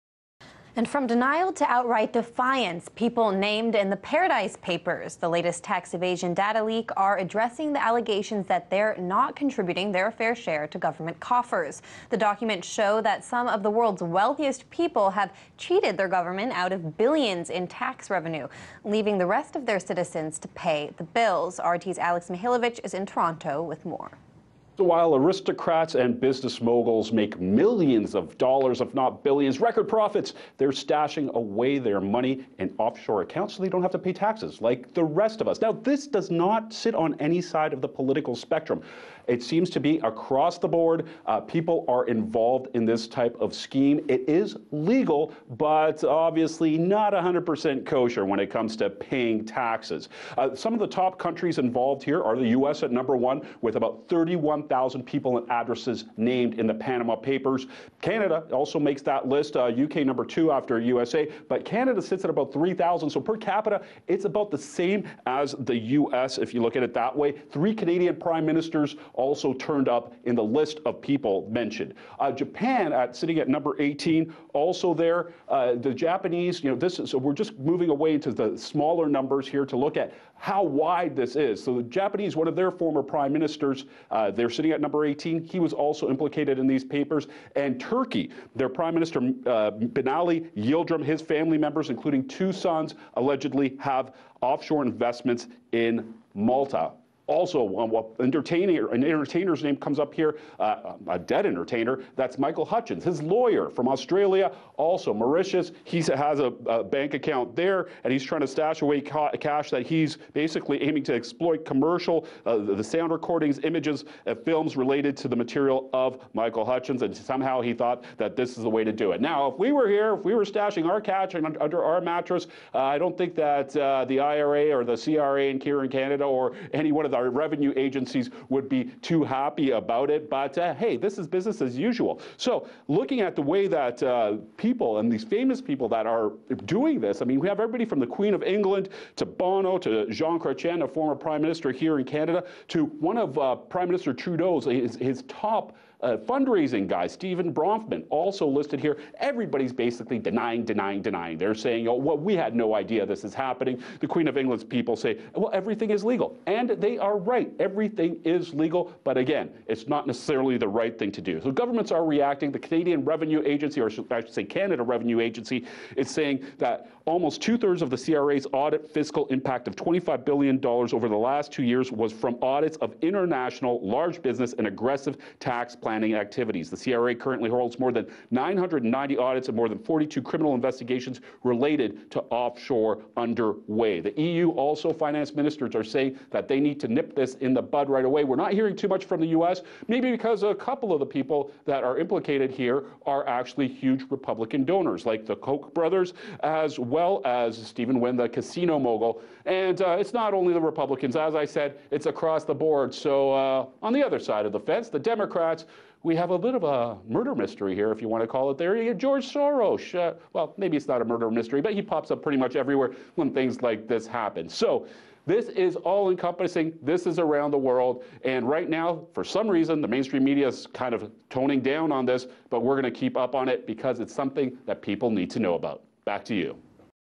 Paradise Papers; World's Wealthiest Citizens Have Cheated Governments Out Of Billions In Taxes
November 07, 2017 - From denial to outright defiance, people named in the latest tax-evasion document leak known as the “Paradise Papers” are addressing accusations that they’re not paying their fair share to government coffers. RT America’s Alex Mihailovich reports on the details of the document dump.